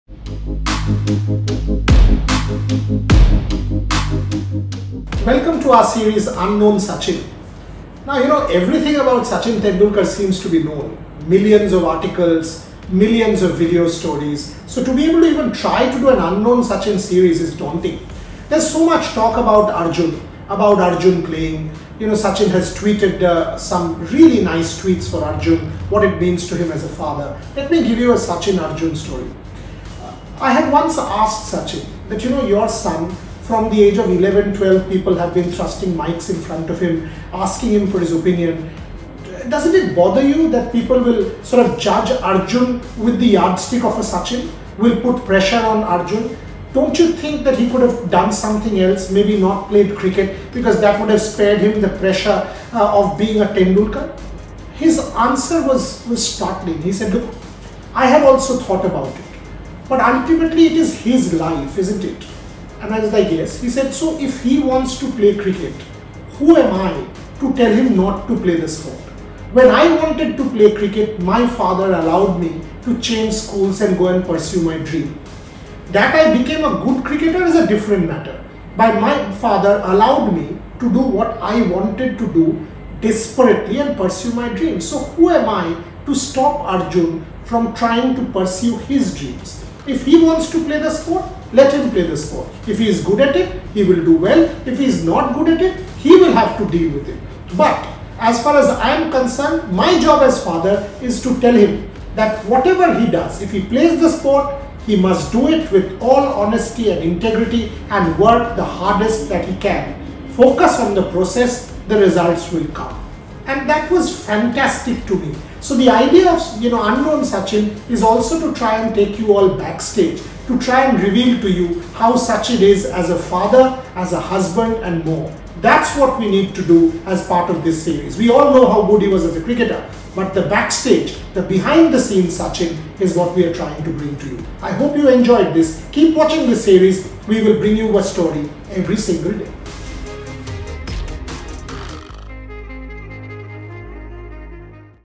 Unknown story of Sachin and Arjun Tendulkar
UnknownSachin - A series where we talk about the SachinTendulkar that is unknown to the World!
Let’s start with a father-son story. How did the Master react when Arjun decided to pursue cricket?